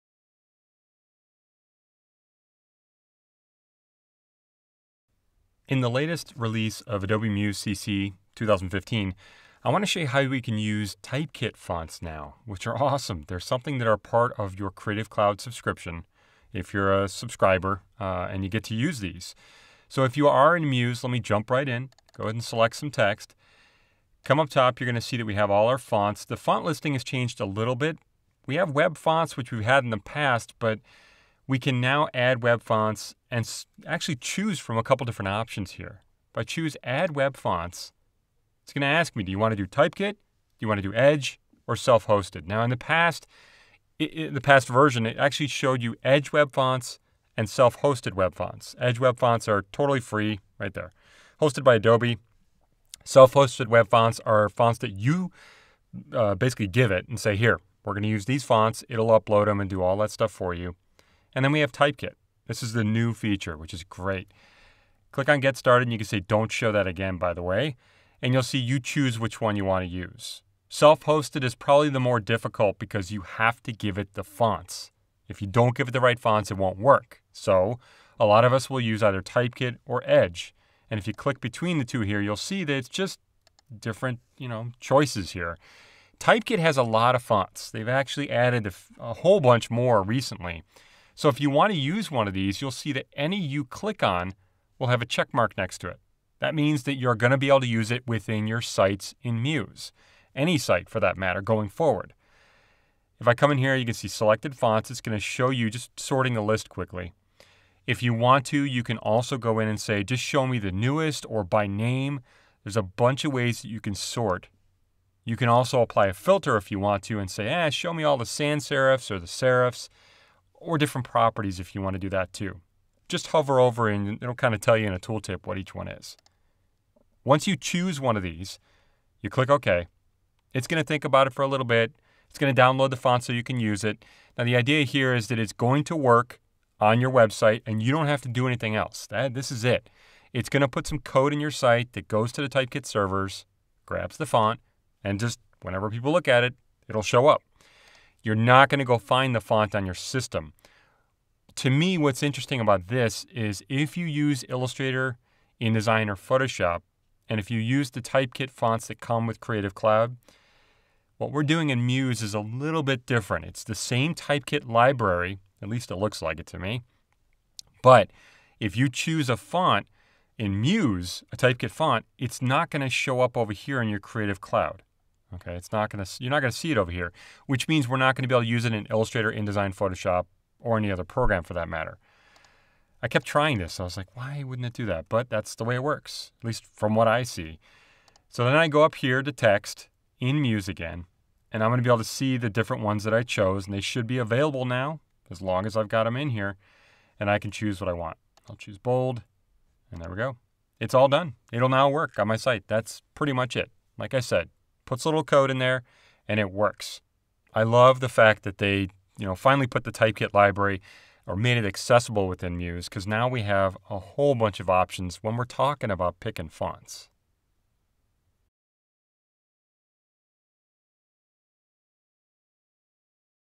Typekit Fonts in Adobe Muse CC (2015)
BrianWoodTraining.com—In Adobe Muse CC, you can now use your Typekit library of fonts. This means you can choose a Typeset font within Muse, and the code will be added to your site so the font just works. Pretty easy.